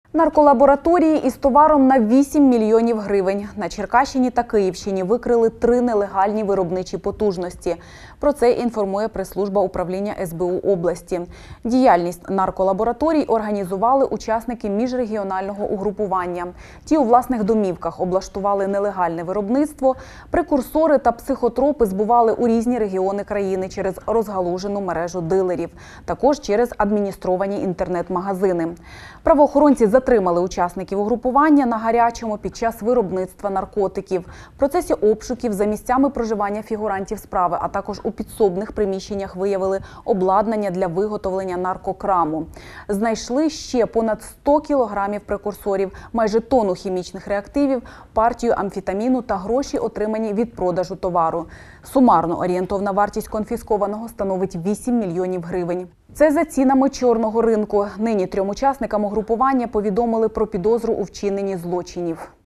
Мільйонний товар: на Черкащині й Київщині викрили три нелегальні «виробничі наркопотужності»
Орієнтовна вартість конфіскованого становить вісім мільйонів гривень